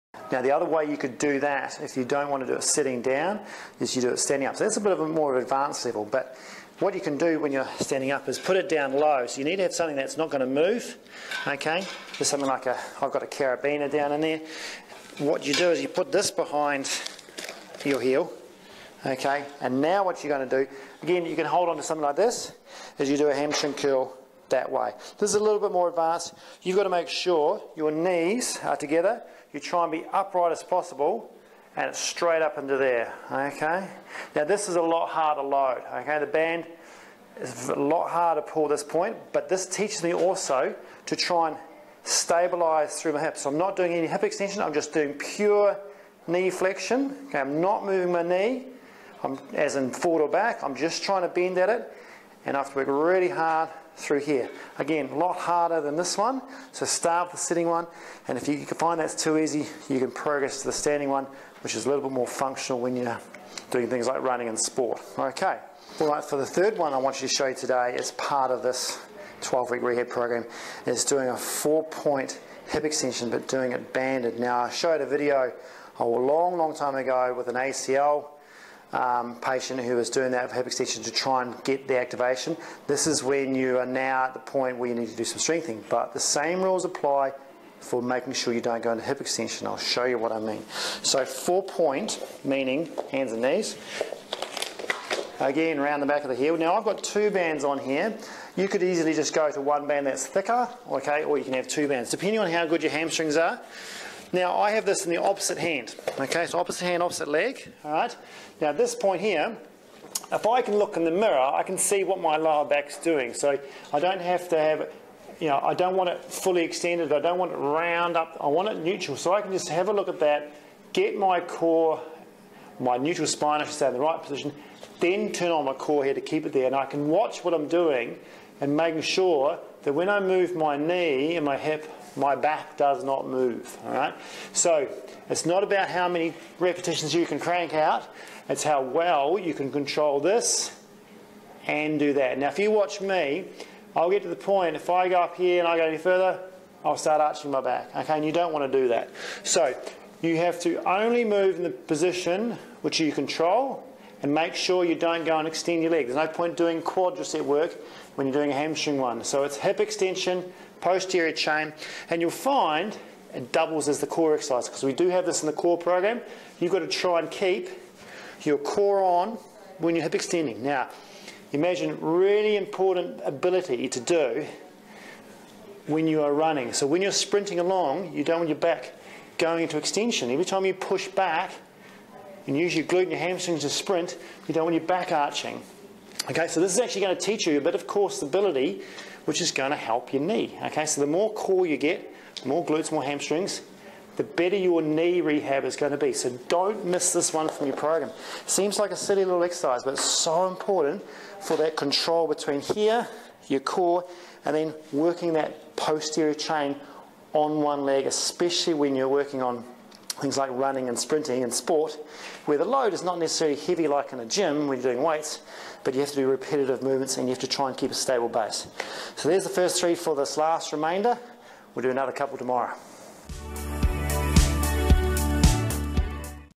Ham curls + Hip Extension - 12 Wk Knee Program: Rehab Ex 11+12 | No.191 | Physio REHAB
1) An advancement of the hamstring curls in sitting - if you can’t do it sitting down - do it standing. ✔️ Bonus with the standing is that you have to also work in some hip and pelvic stability - which leads into
2) 4-point hip extension (banded) which guess what, also doubles as a core strength and stability exercise (coz we all need it!!!) which really helps you with your knee strength because if you have better core awareness on hip extension / posterior chain then your will do better with the hamstring and hip extension strength. 👍Make sure you don’t omit this exercise and get it right by using mirrors and watching your lumbar spine neutral.

To check out the free weeks preview section that we are showcasing for the KNEE + PATELLO-FEMORAL REHAB 12 WEEK PROGRAM, simply go to: ➕